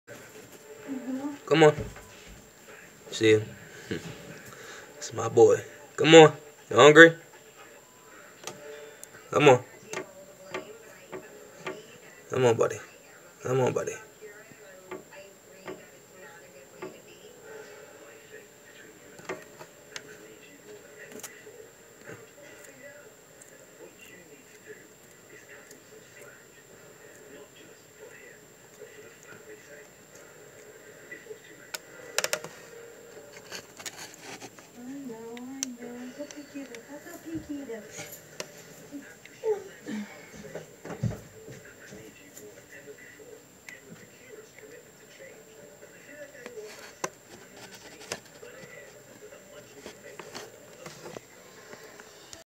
Feeding Cocaine Cow Boy
Showing You How He Looks And Acts When He Gets Hungry Lol. I Like Feeding Him .